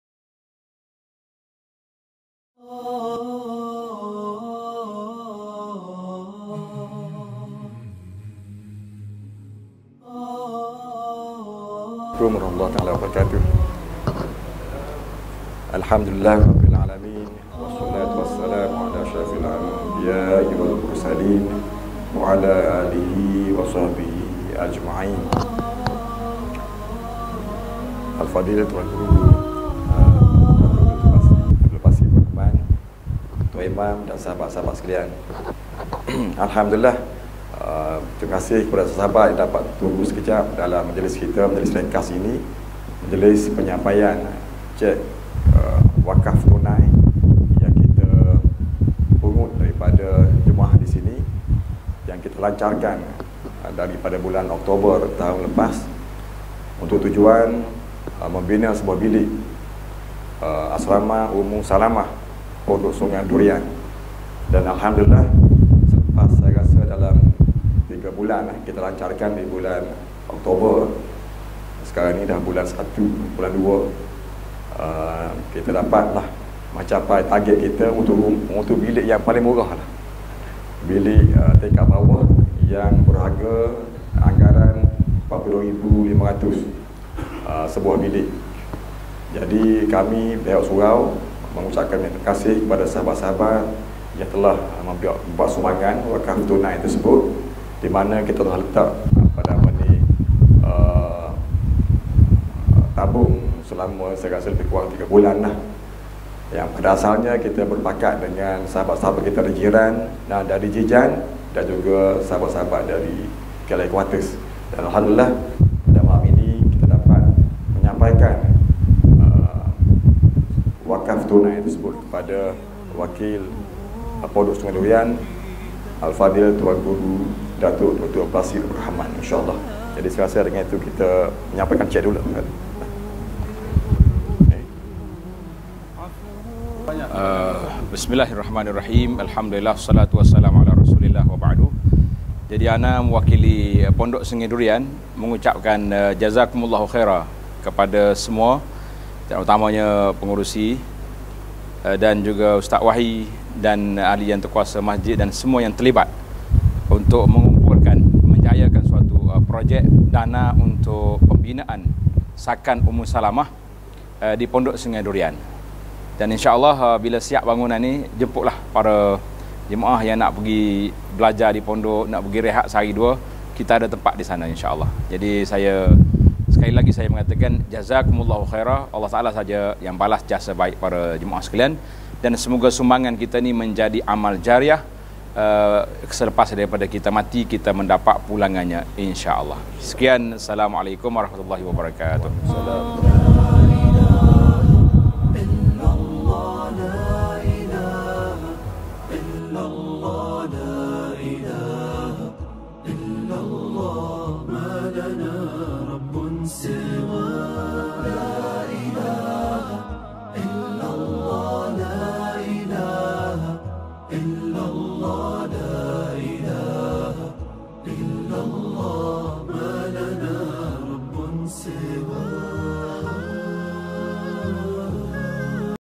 Majlis Penyampaian Cheque Wakaf Tunai Wakaf Ummu Salamah
Surau al-Islah, Kota Seri Emas, Nilai